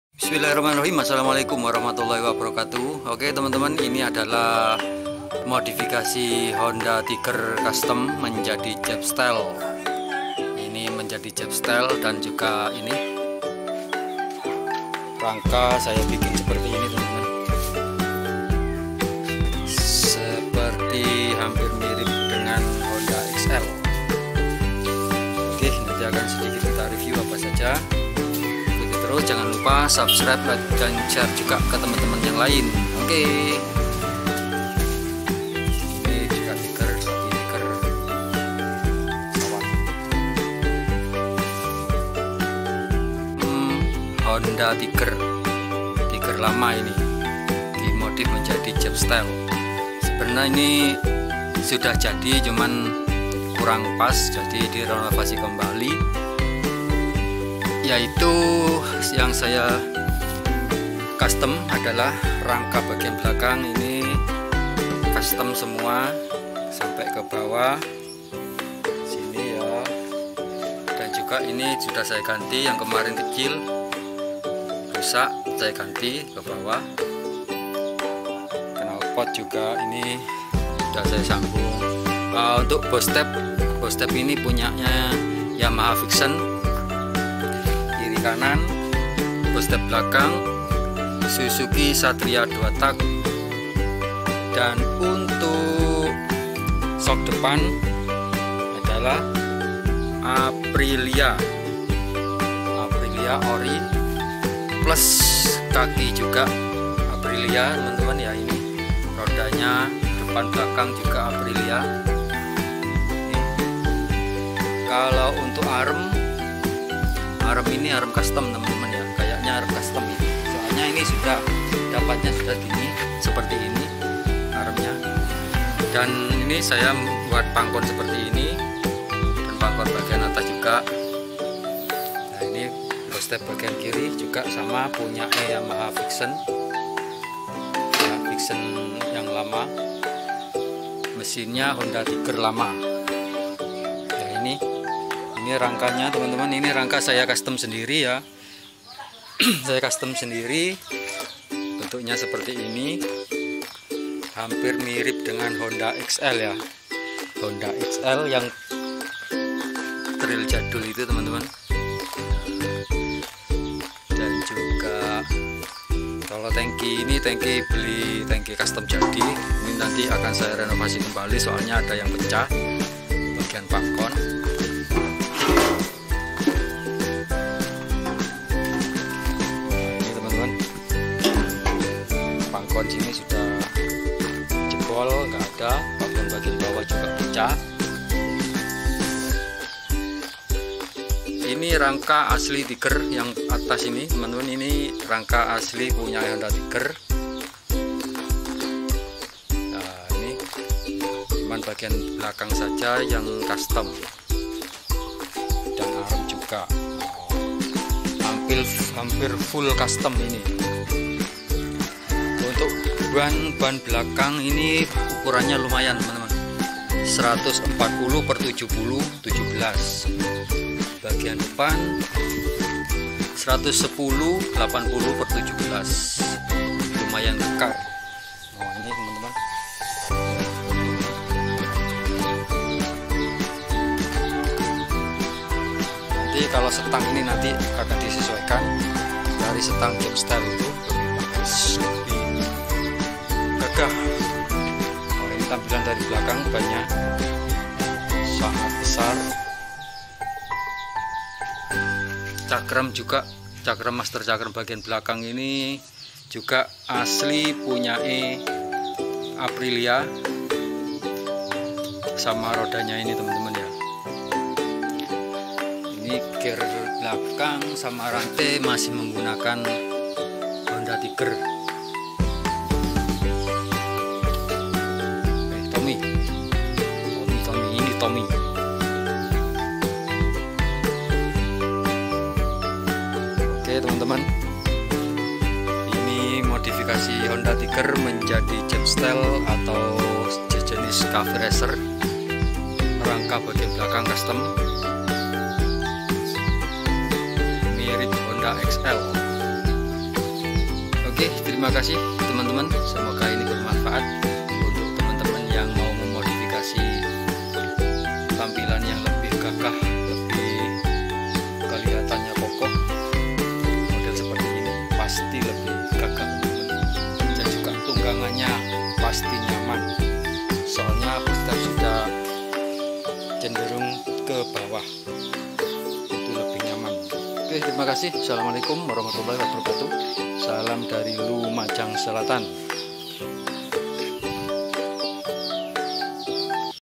Modifikasi Honda Tiger jadi jabstyle milik Youtuber
Modifikasi Honda Tiger jadi jabstyle ini adalah salah satu motor custom milik Youtuber @bangarik212  motor yg minta untuk di restorasi kembali supaya keren..